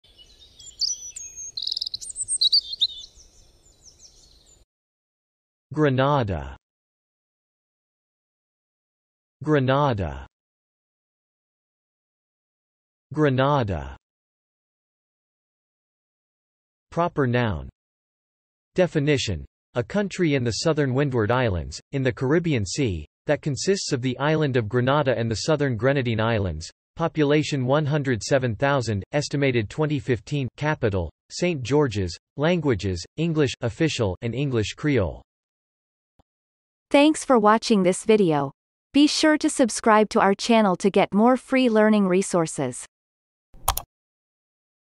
How to pronounce grenada - Vocab Today
The pronunciation of the word grenada in Amercian accent is demonstrated in this video. If the word is from another language, such as brand name, it will be pronounced in its original language. Based on the word frequency, this is of 3 stars difficulty.

The word grenada has the following meanings:
1. (proper noun)
Definition : A country in the southern Windward Islands, in the Caribbean Sea, that consists of the island of Grenada and the southern Grenadine Islands; population 107,000 (estimated 2015); capital, St. George's; languages, English (official) and English Creole.